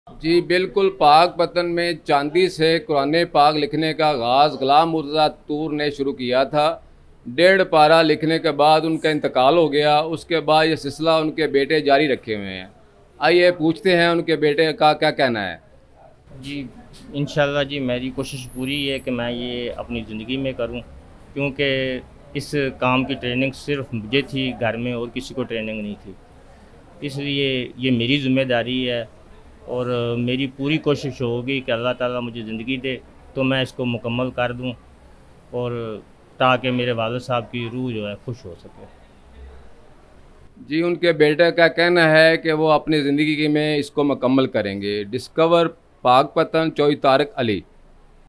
Chandi Ke work Per Quran Pak Ke Haroof | Baap Ke Bad Batey Ne Zamindari Sambhal Li | Pakistan Today
Chandi Ke work Per Quran Pak Ke Haroof | Baap Ke Bad Batey Ne Zamindari Sambhal Li